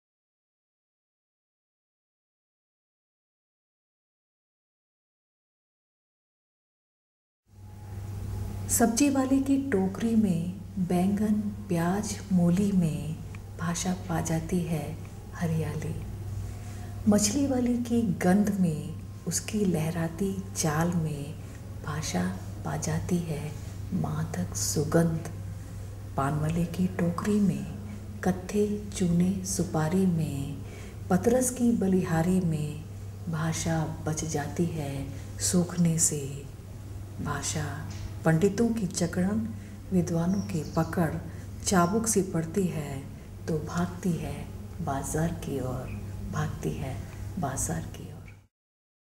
rati language
a poem language by Rati Saxena- Hindi poet about journey of the language recorded by Odveig Klyve